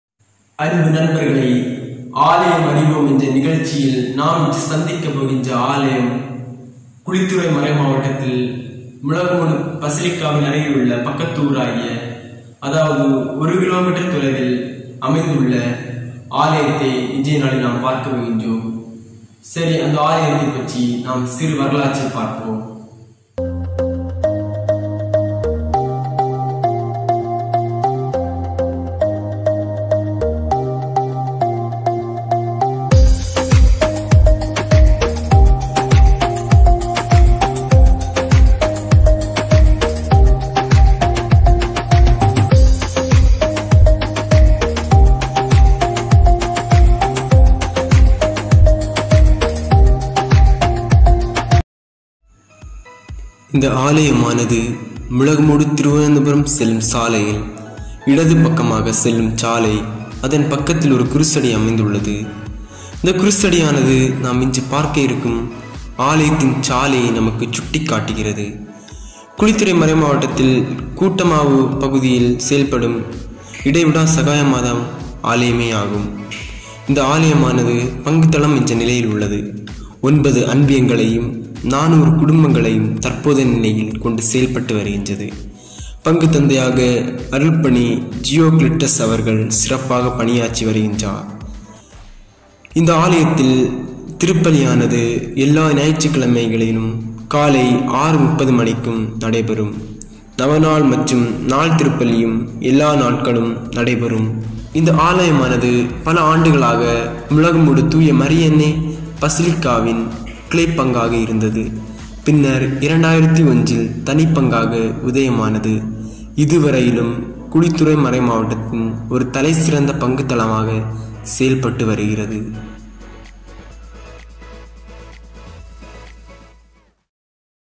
Aalayam Arivom |Episode-01| ஆலயம் அறிவோம் |koothamavu Parish |Kuzhithurai Diocese |Kanyakumari
நாம் இன்று சந்தித்த ஆலயம் குமரி மாவட்டத்தில் கத்தோலிக்கர்களிள் மறைமாவட்டமாக செயல்படும் குழித்துறை மறைமாவட்டத்தின் கீழ் செயல்படும் கூட்டமாவு பங்கை பற்றி சிறு குறிப்பை  ஆலயம் அறிவோம் நிகழ்ச்சியில்  பார்ப்போம்.

அனைவரும் இந்த யூடியூப் சேனலுக்கு முழு ஒத்துழைப்பு தாருங்கள்.